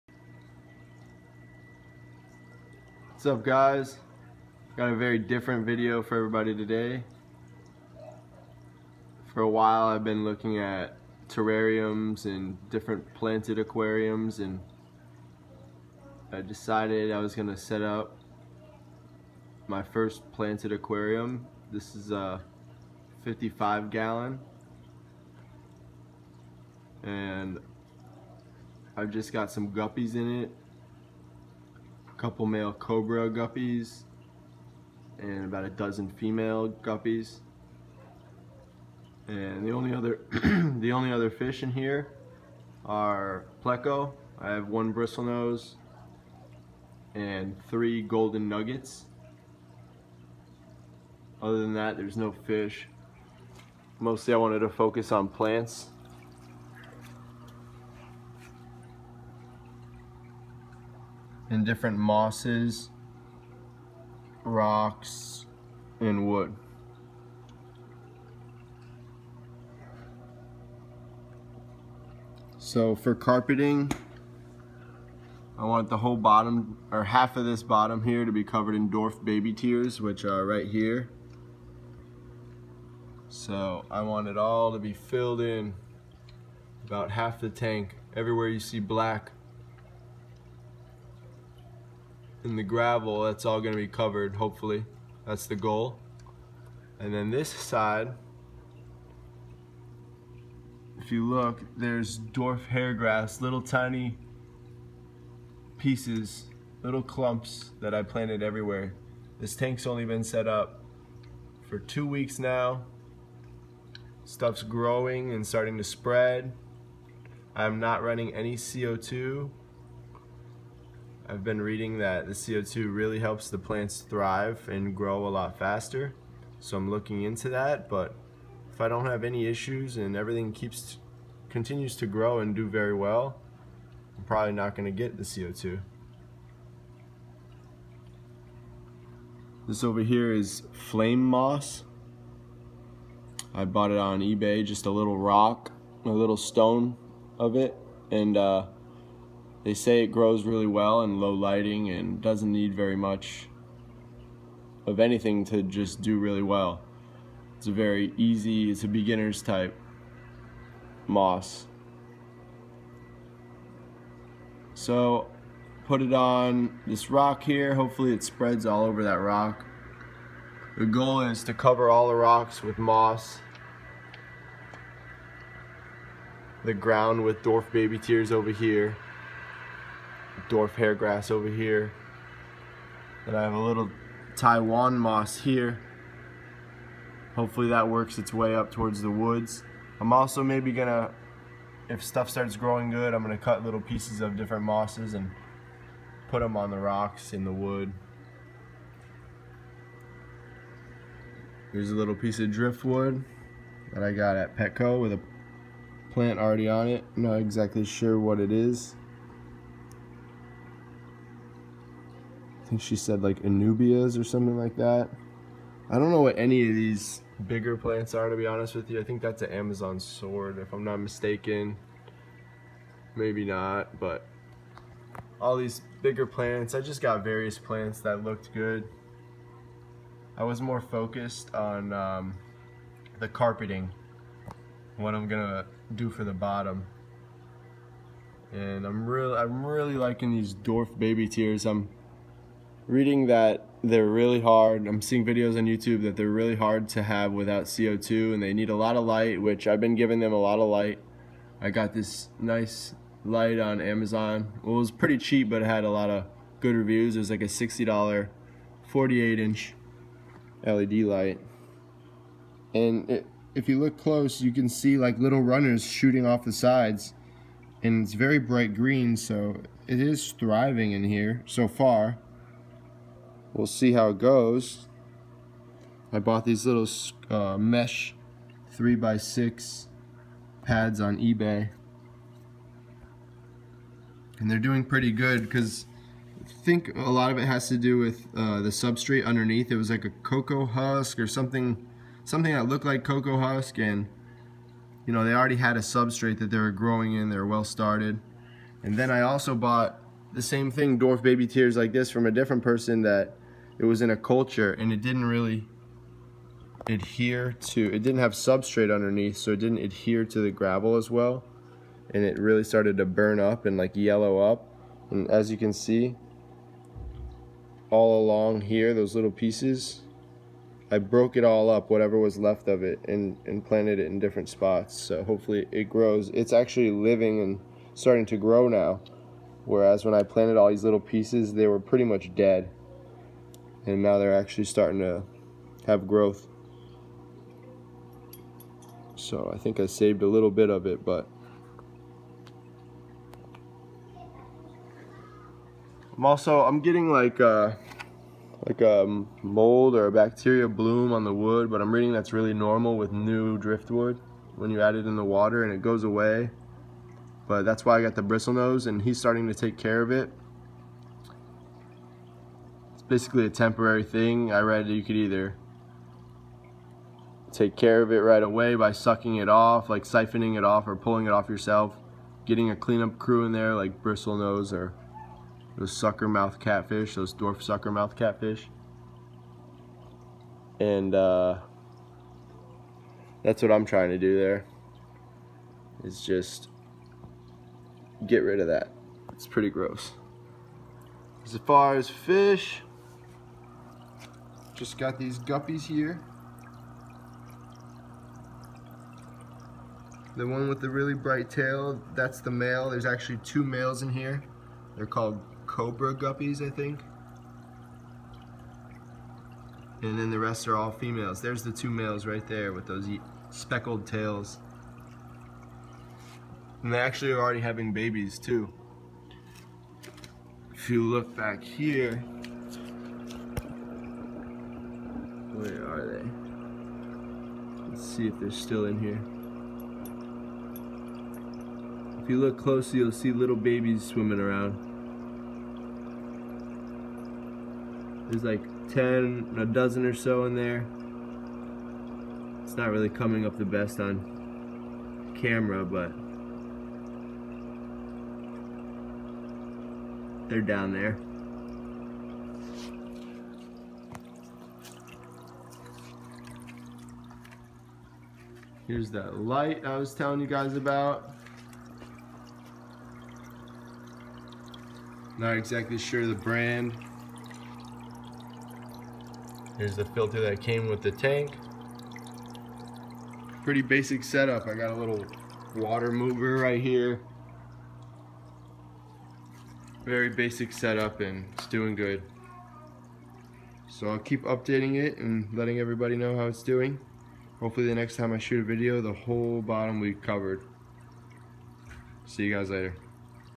My First Planted Aquarium!
Here’s my 55 gallon planted tank. Enjoy.